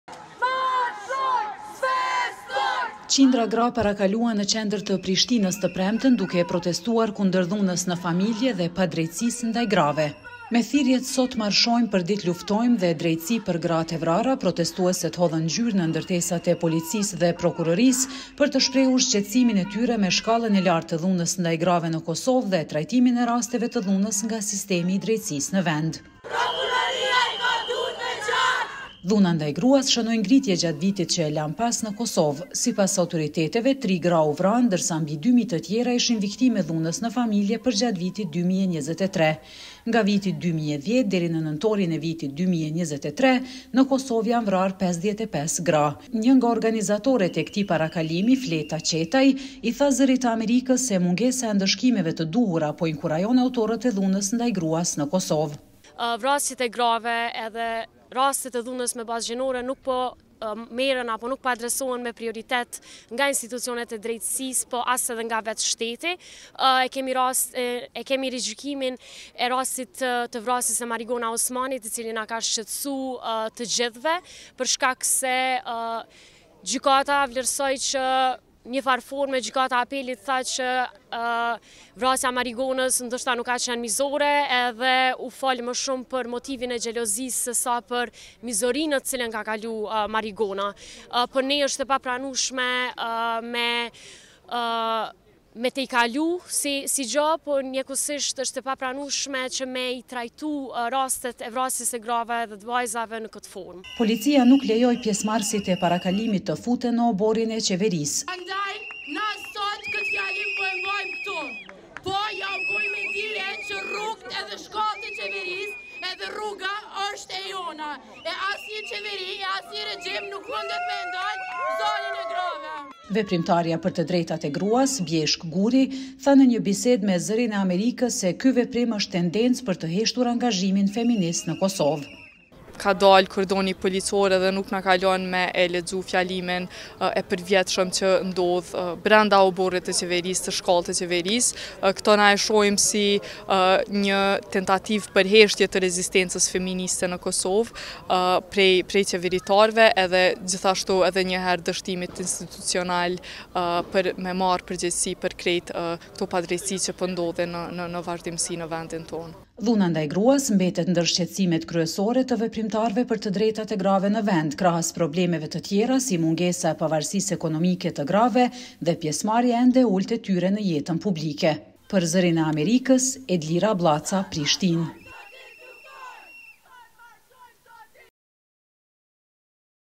Kosovë, 8 marsi shënohet me thirrje për drejtësi dhe barazi për gratë
Në Kosovë, Dita Ndërkombëtare e gruas u shënua të premten me një parakalim ku veprimtarët kërkuan trajtim më të mirë të rasteve të vrasjes dhe dhunës ndaj grave nga sistemi i drejtësisë në vend